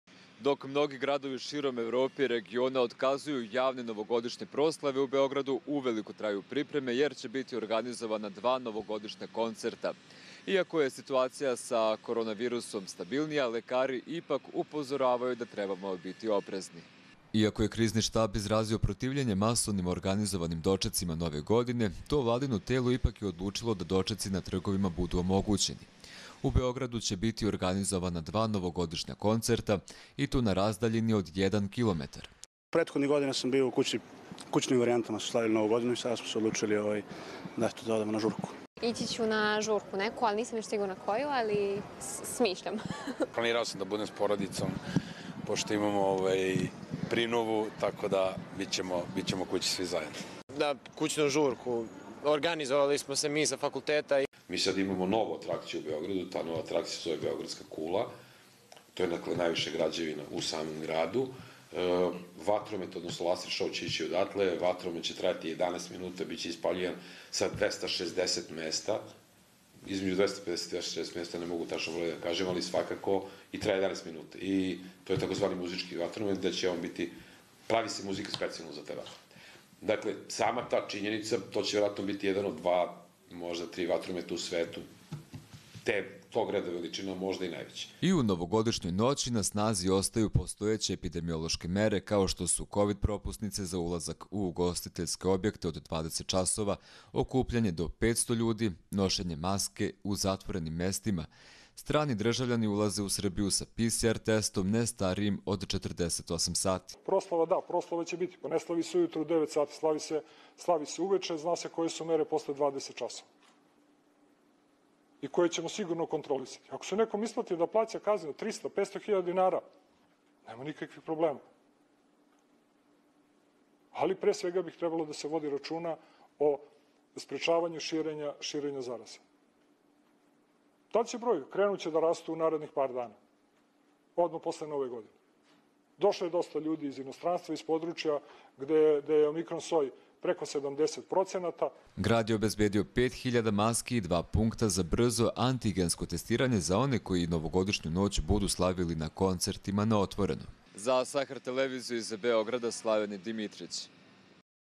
U Beogradu dva novogodišnja koncerta
Unatoč epidemiji koronavirusa, u Beogradu će biti održana dva novogodišnja koncerta. Iz Beograda izvještava Slaven Dimitrić.